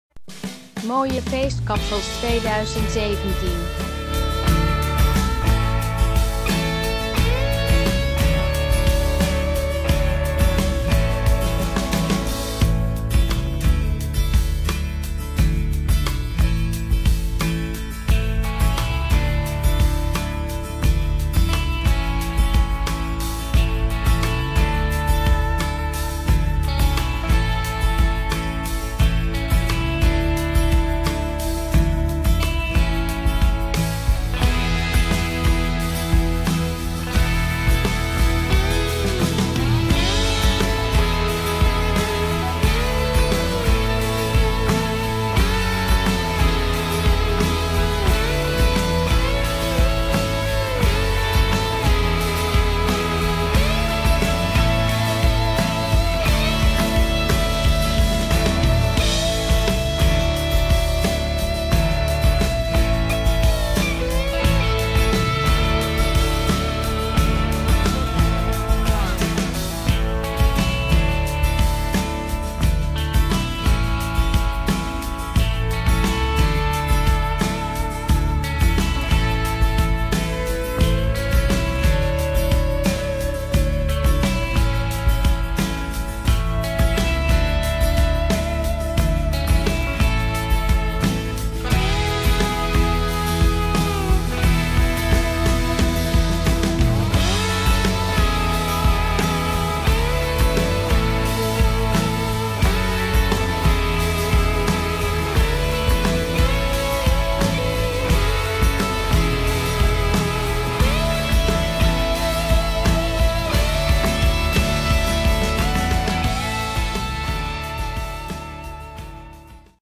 Mooie feestkapsels 2017
De video toont mooie feestkapsels 2017.

Wilt u een kapsel te maken in 2017? Kies de meest moderne kapsels voor vrouwen in 2017.